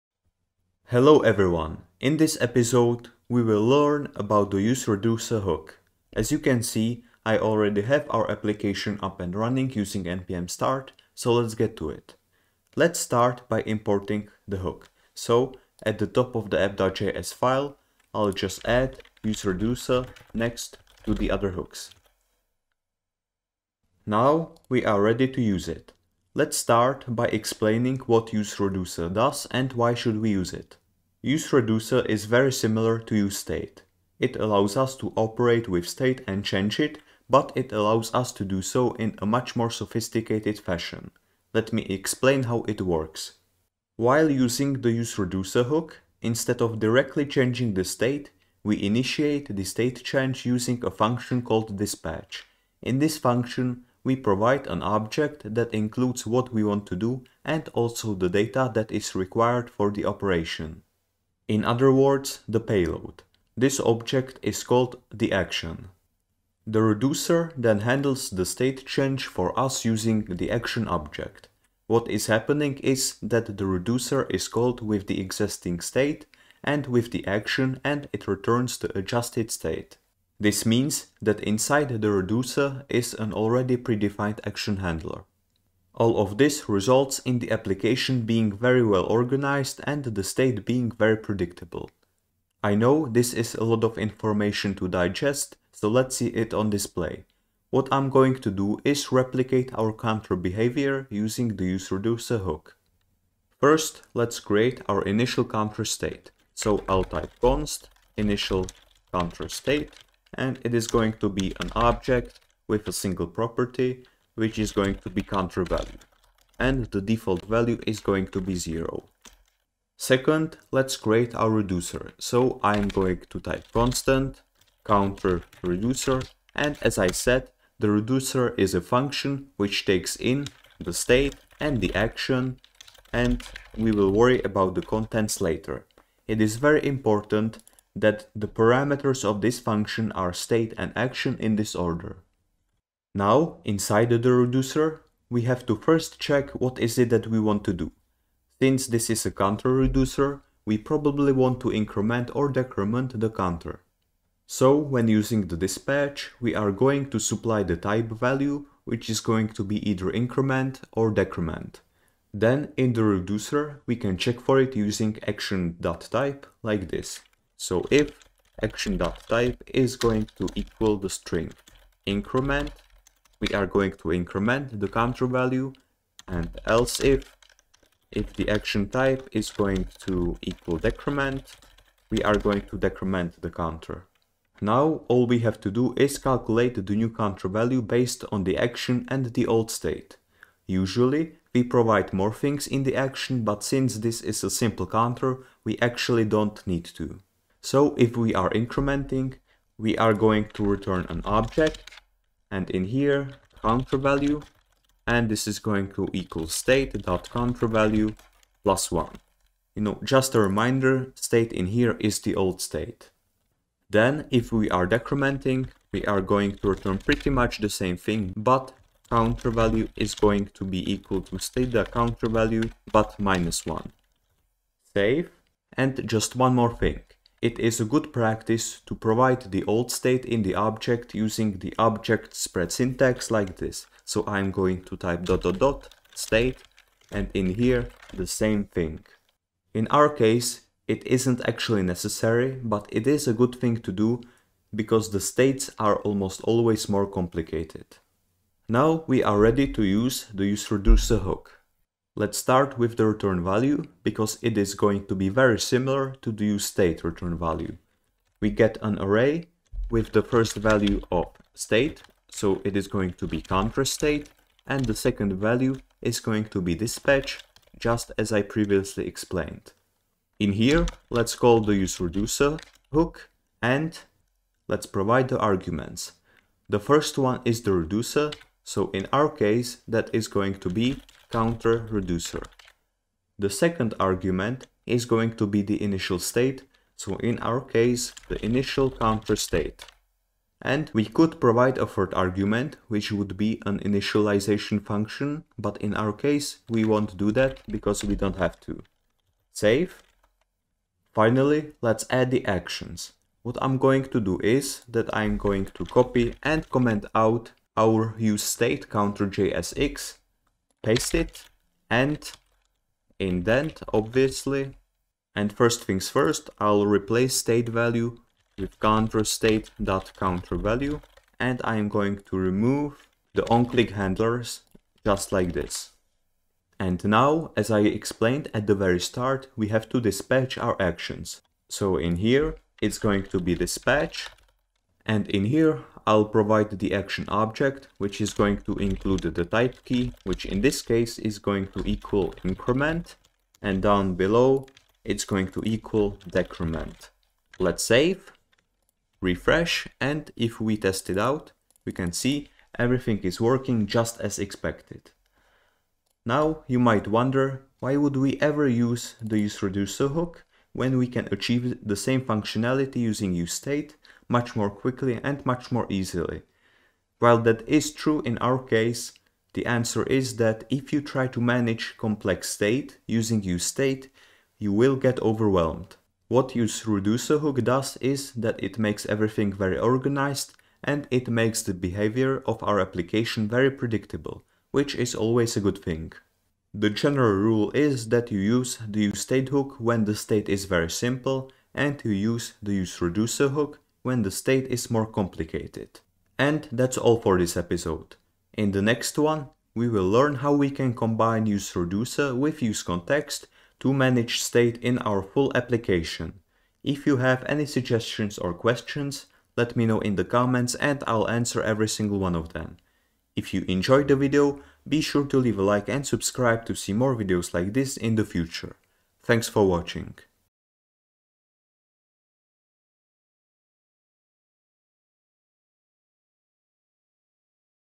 useReducer | React Hooks Tutorial #6
In this episode of my React Hooks Tutorial, we will dive into the useReducer hook and explore, how to use it. We will learn about the dispatch function as well as the action object. We will also learn how does useReducer differ from useState and why we should use it in more complex applications.

Timestamps:

►0:00 Introduction
►0:12 Importing the useReducer hook
►0:25 Explaining how useReducer works
►1:43 Creating reducer with initial state
►2:22 Creating action handlers
►4:08 Calling the useReducer hook
►5:00 Dispatching actions
►6:36 Ending